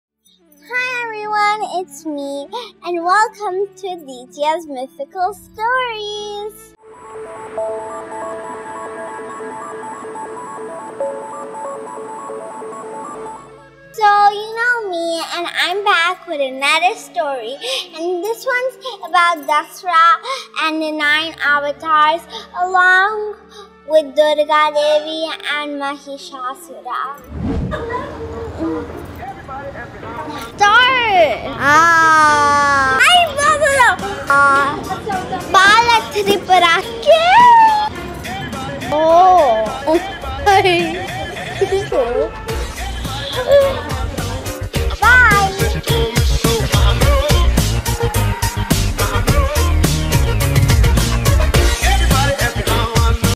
The Story of Goddess Durga in English | Mythical Stories | Dussehra Festival | Deetya
The Story of Goddess Durga in English for Kids
Durga Pooja is widely celebrated in India, in the honour of goddess Durga, who fought and won Mahishasura, to restore peace and dharma in the world.
From then, she is known as Mahishasura Mardini. Watch this interesting story by MagicBox Animations About Mahishasura Mardini on this occasion of Navaratri.

Full Video is coming soon.

Love you all, Stay Home Stay Safe...

Deetya Vlogs,Deetya Gantla,Tickle Kids,Tickle Kid